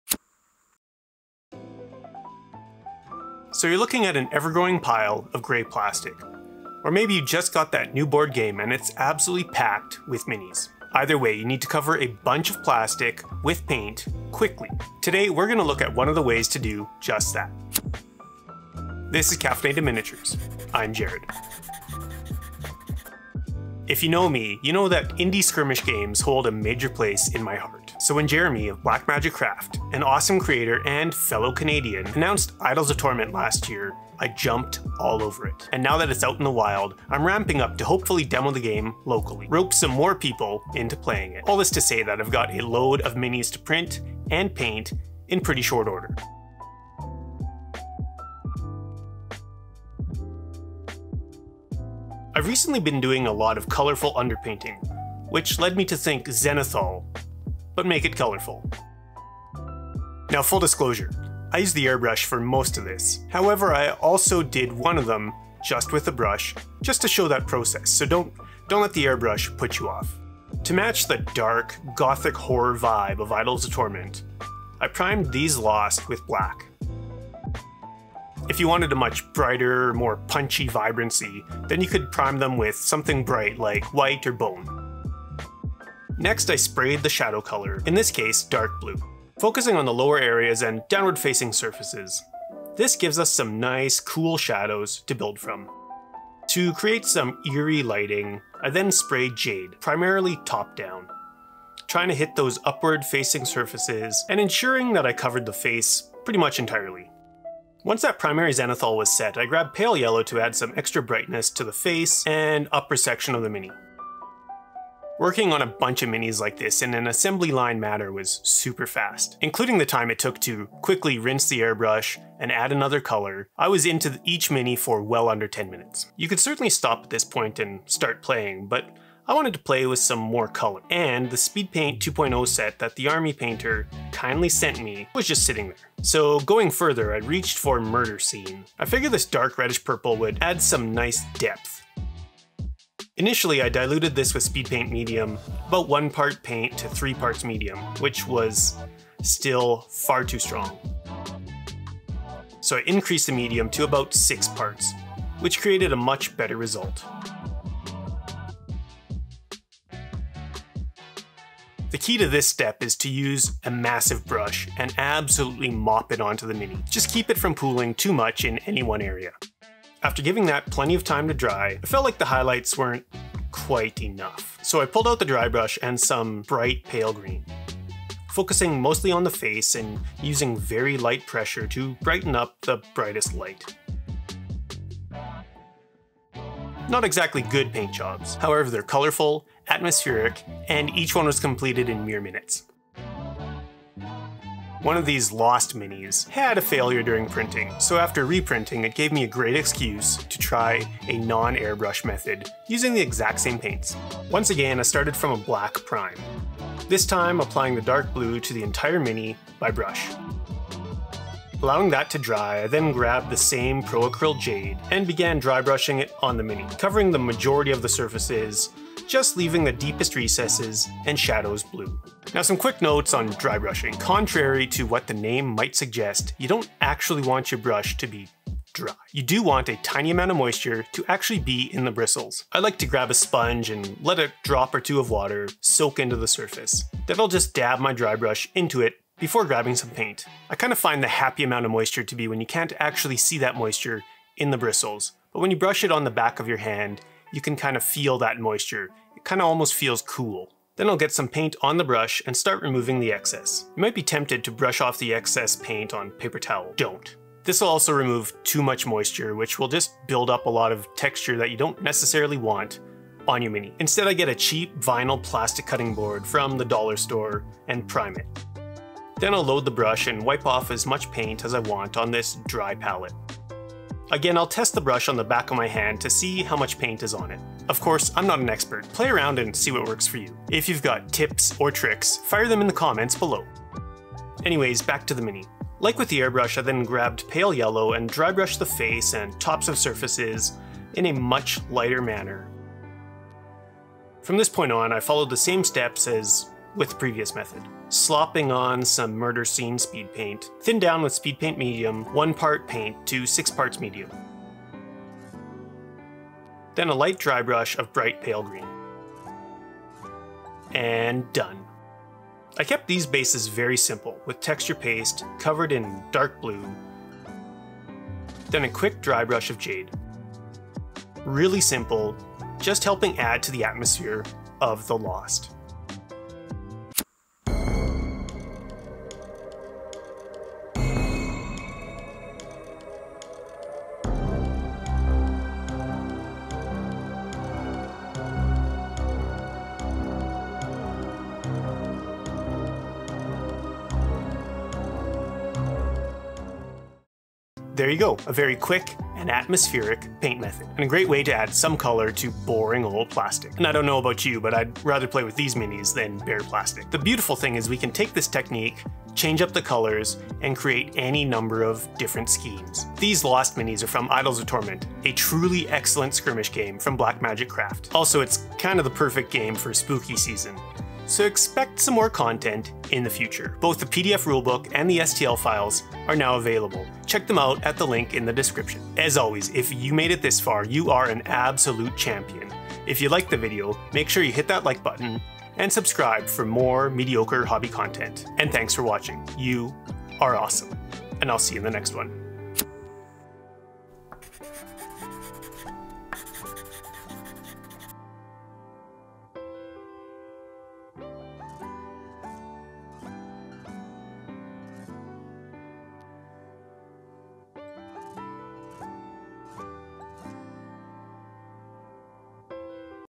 This is the FASTEST Way to Paint Minis for Board Games and Warhammer
I'm getting ready to do some local demos of Idols of Torment...so I needed to come up with a way to paint The Lost models QUICK!!!

Idols of Torment is an incredible tabletop skirmish war game, with a distinct gothic horror vibe, brought to you be Jeremy of @BlackMagicCraftOfficial